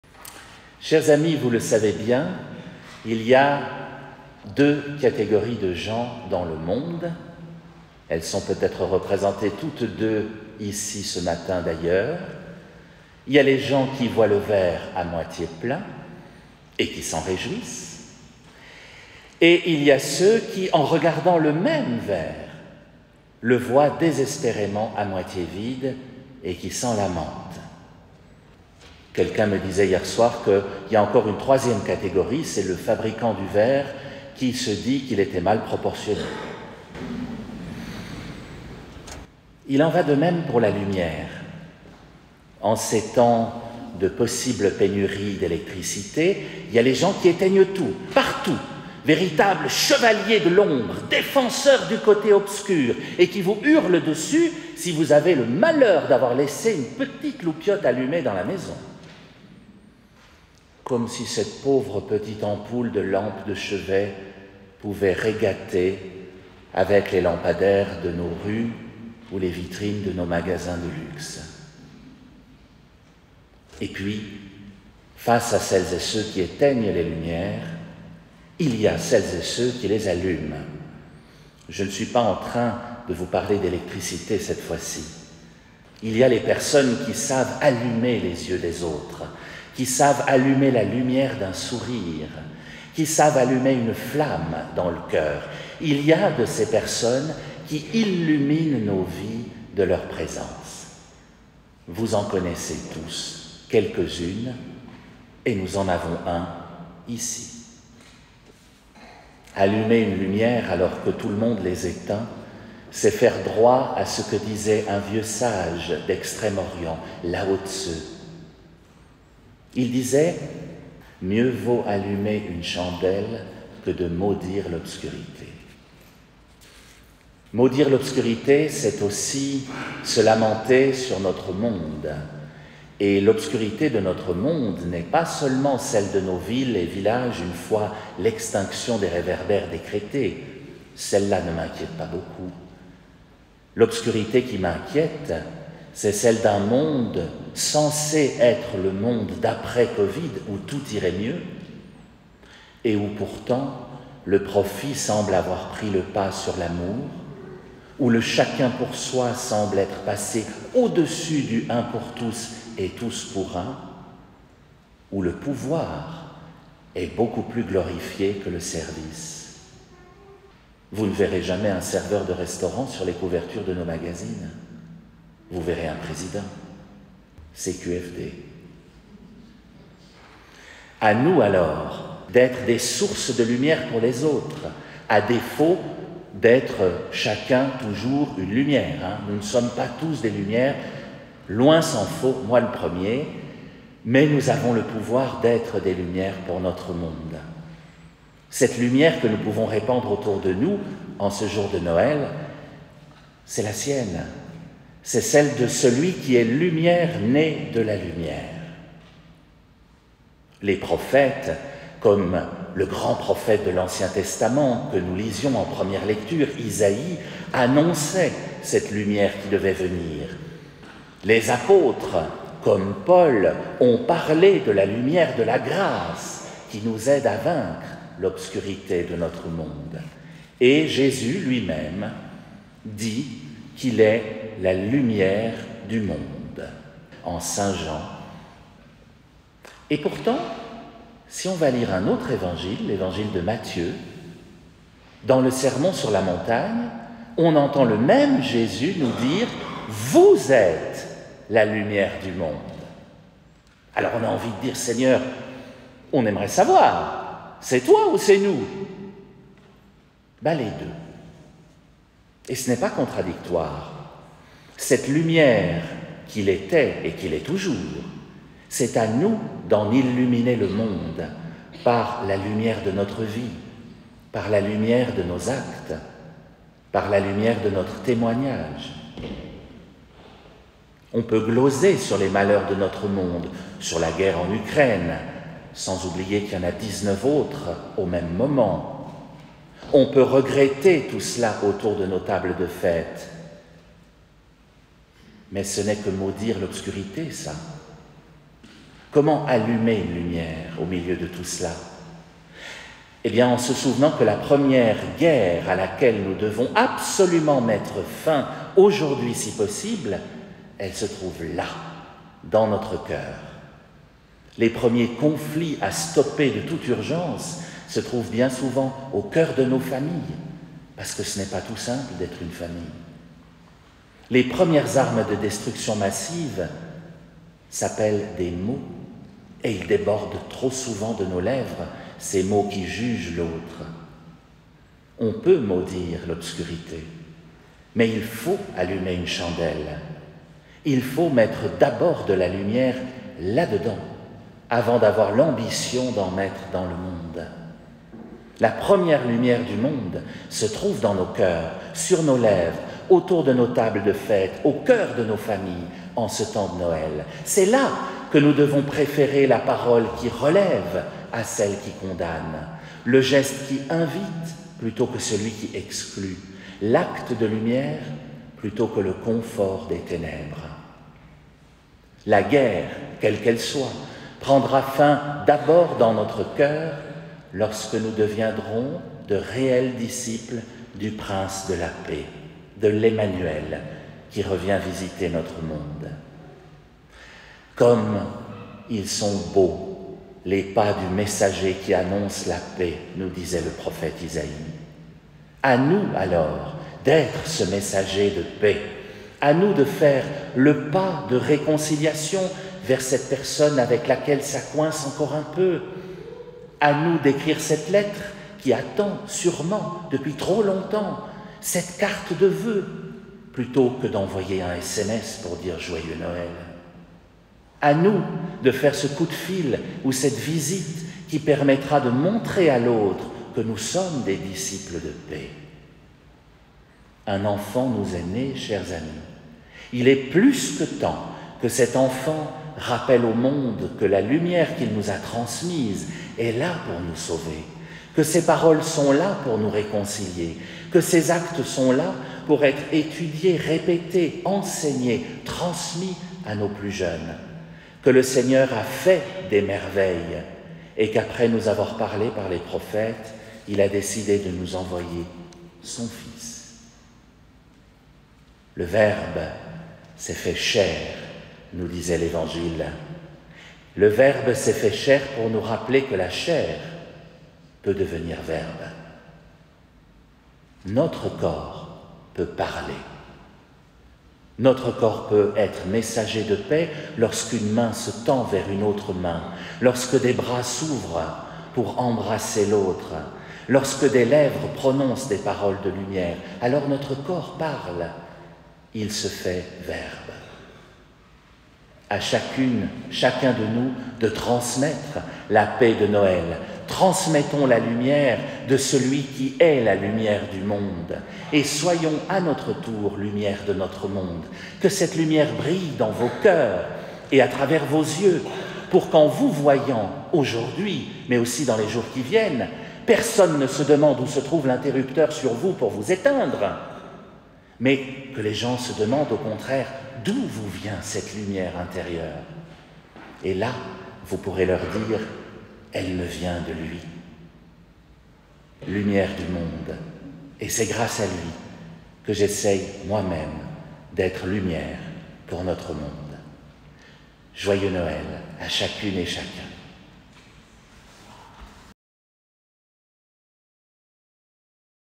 A nous d'être Lumières du Monde
Cette homélie nous rappelle qu'au nom de Celui qui est la Lumière du Monde, nous avons à transmettre cette lumière dans le monde.